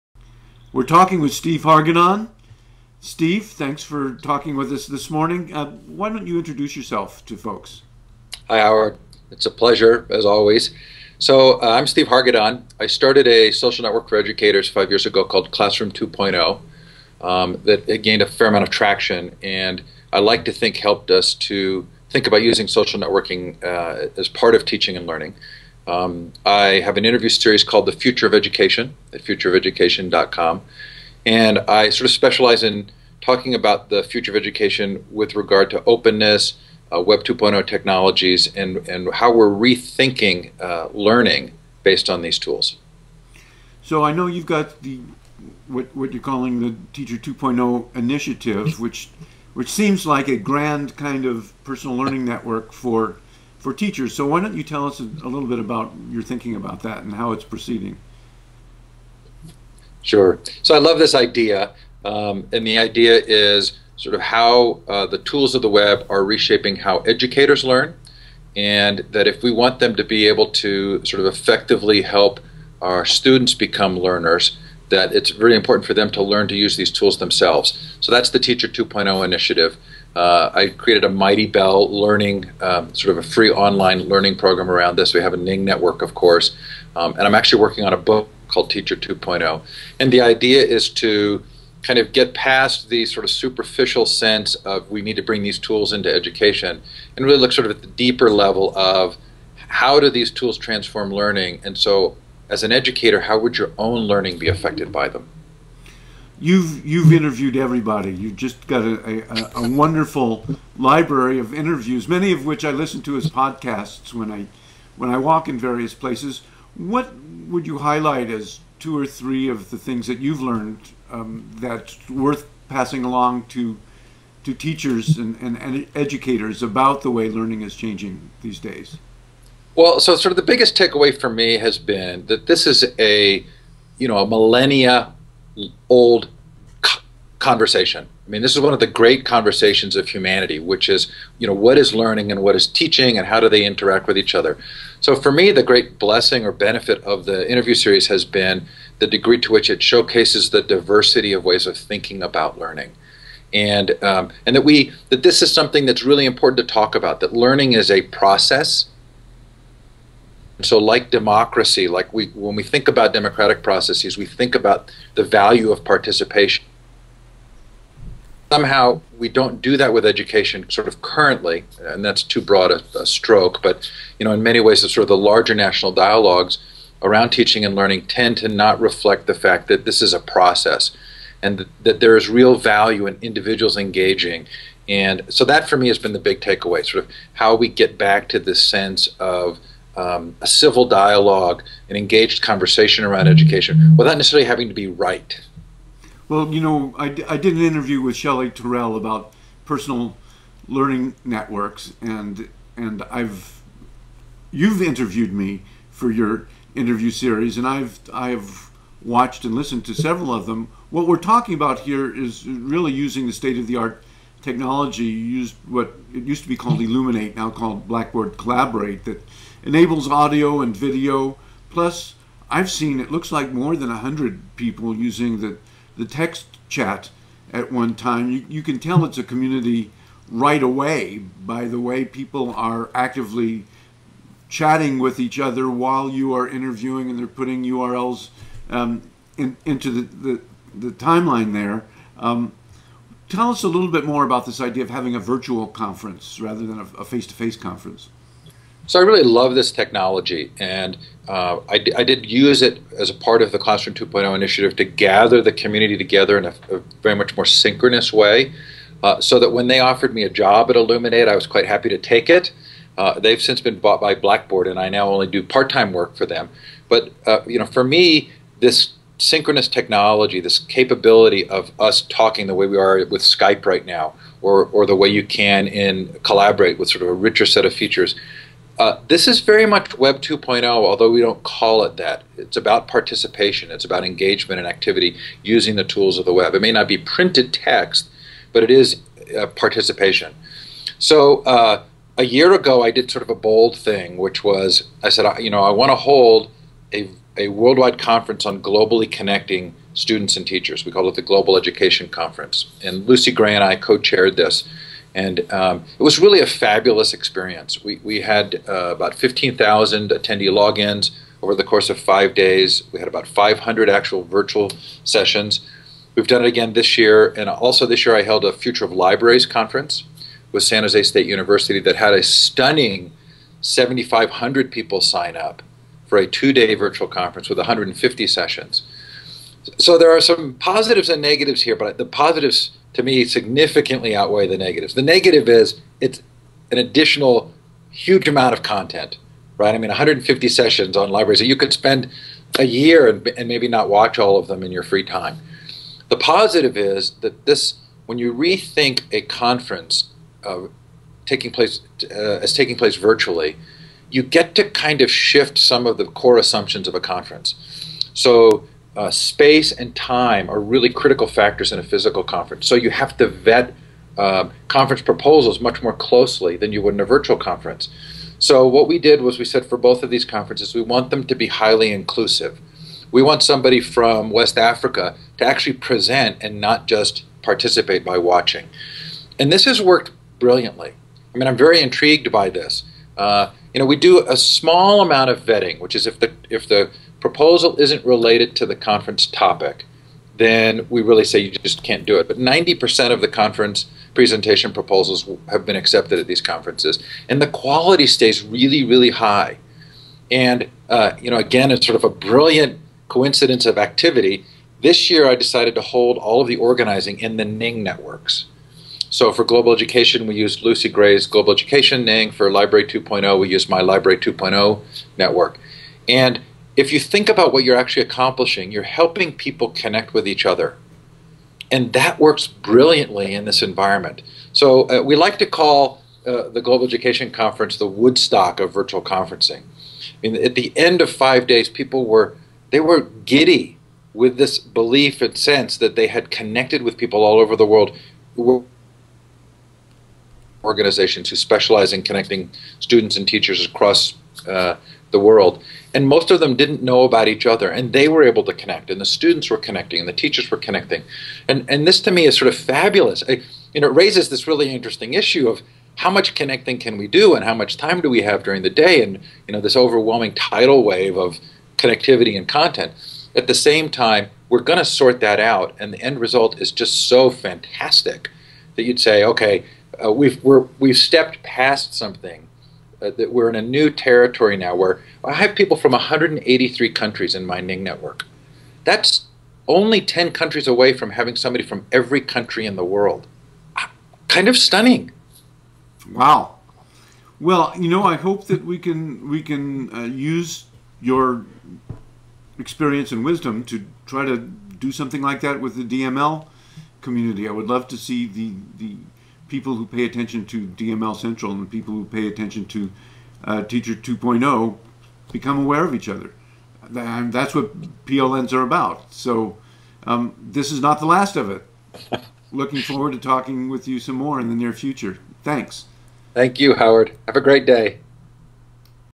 Steve Hargadon: Reimagining Education as Networked, Participatory, Social and Global
Steve Hargadon is the Charlie Rose of technology, learning and teaching. On his website, the Future of Education he has interviewed everyone: from Clay Shirky to Diane Ravitch to Ken Robinson to Howard Gardner, and nearly everyone in between.

He's been at the center of open education resources, Web 2.0, and social networking for as long as anyone. He is a master of virtual live events. is essentially a megasized personal learning network for teachers.

In this video, crowdsourcing pioneer Howard Rheingold talks with Steve about "rethinking learning."